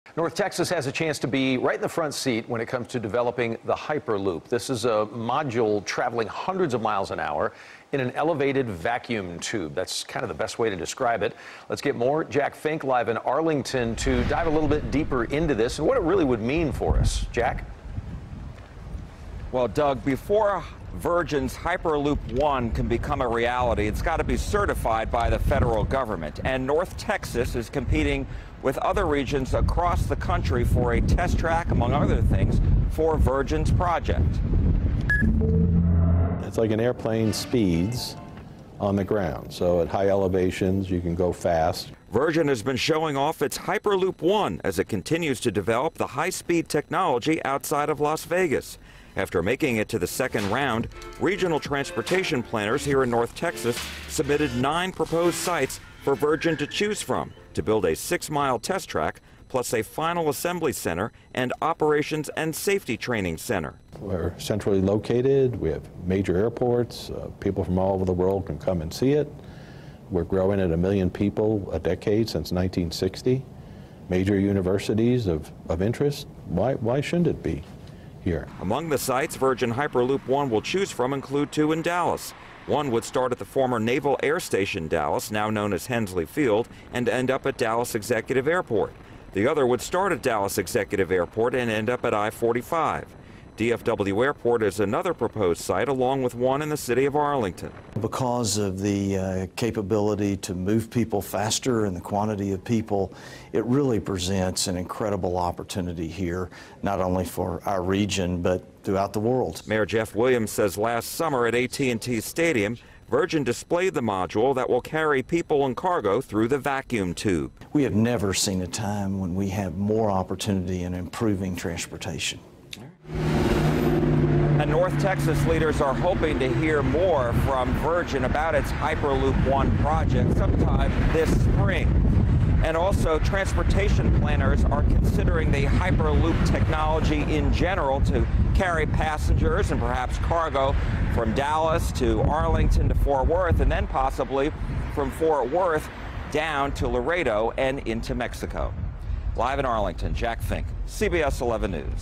North Texas Could Play Role In Developing Hyperloop
Before it can become a reality, Virgin's Hyperloop One, must be certified by the federal government. And North Texas is competing with other regions across the country for a test track for Virgin's project.